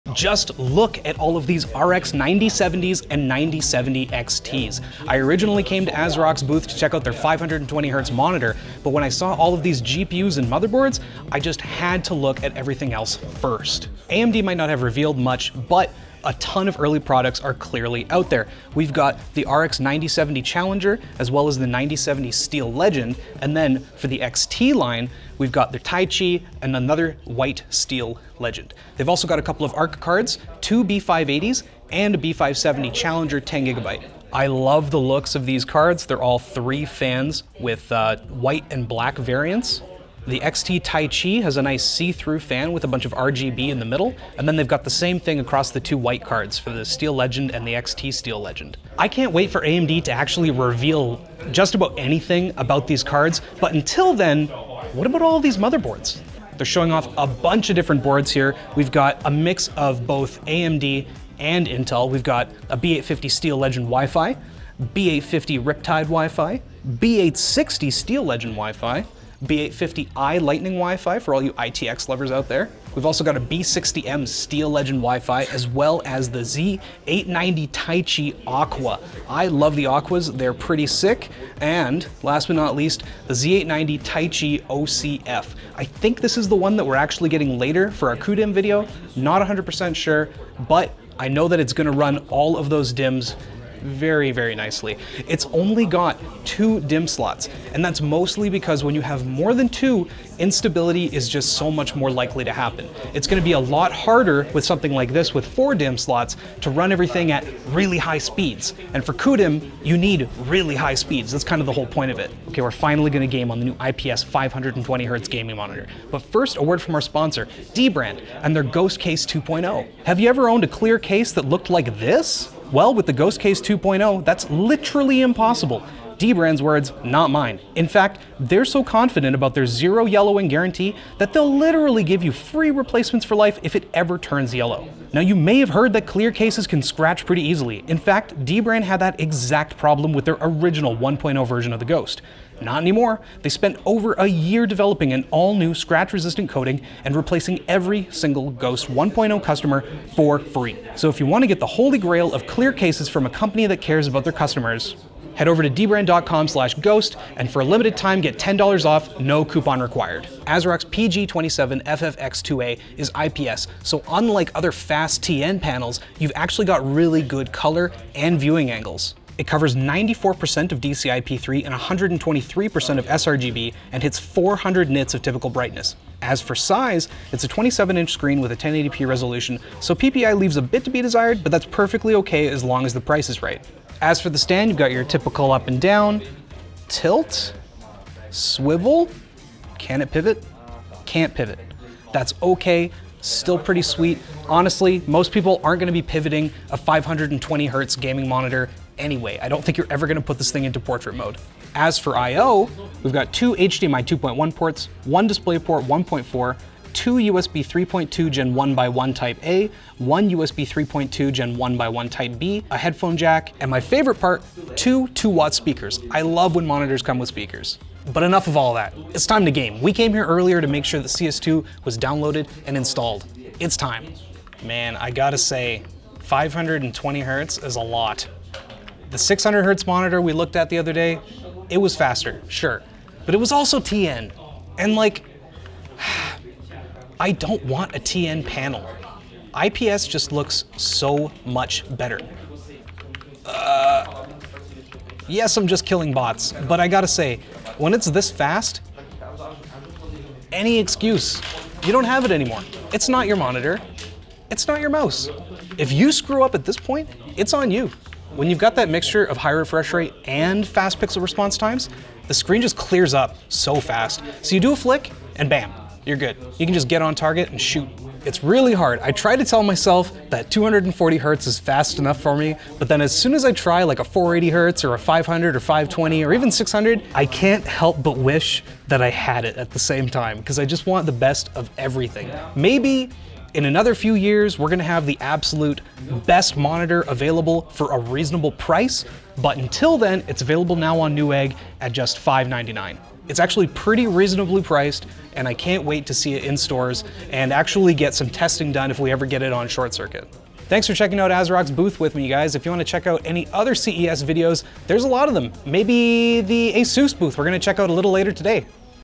I Found AMD's New RX 9070 Graphics Cards! - ASRock at CES 2025
We stopped by the ASRock booth to see their new 520Hz monitor, but then we stumbled upon something even more interesting... the New RX 9070 graphics cards! With NVidia stealing the spotlight, we took a closer look at the new AMD GPUs and ASRock's new monitors and motherboards.

CHAPTERS
0:00 AMD RX 9070 GPUs
0:57 Motherboards
2:00 Sponsor - dbrand
2:40 PG27FFX2A 520Hz Monitor
5:00 Outro